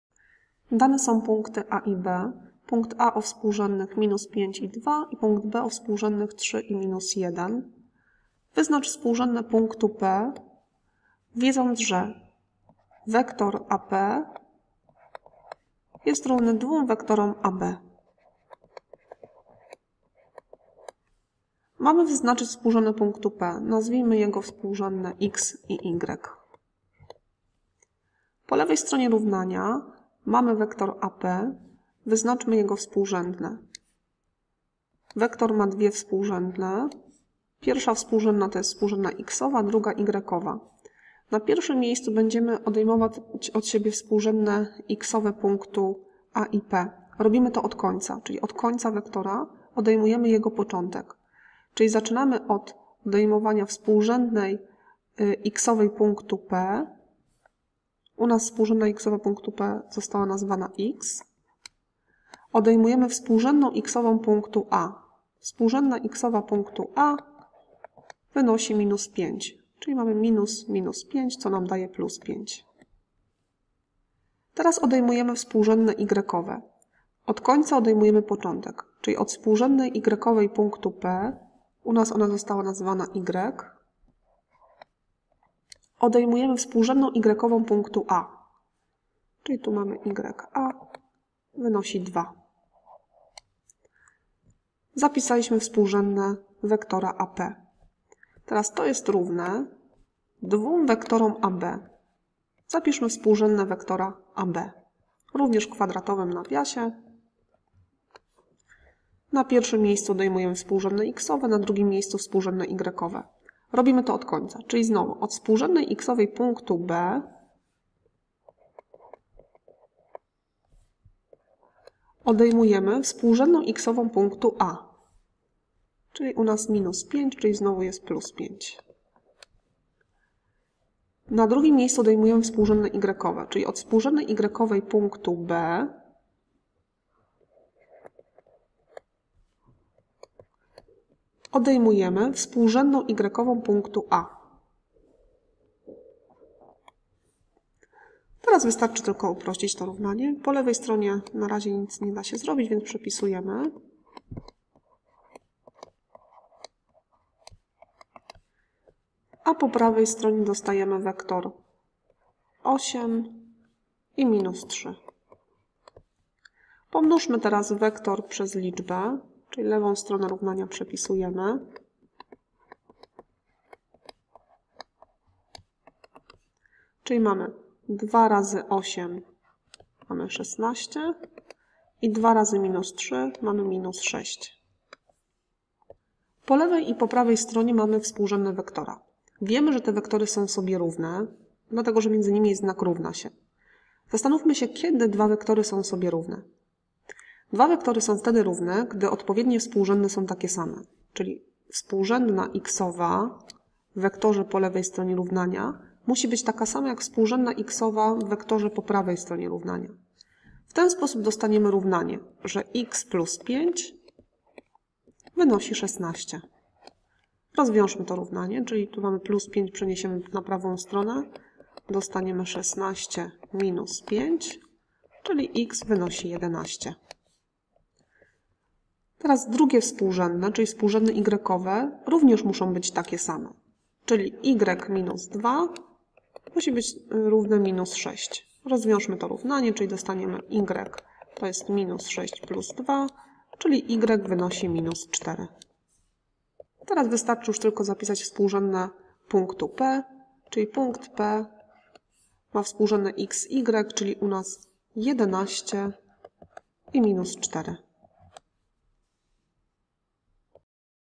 Działania na wektorach
Dane są punkty A i B. Wyznacz współrzędne punktu P wiedząc, że AP=2AB

Film powstał jako pomoc do kart pracy, które znajdują się na stronie w zakładce: MATMA: GEOMETRIA ANALITYCZNA: DZIAŁANIA NA WEKTORACH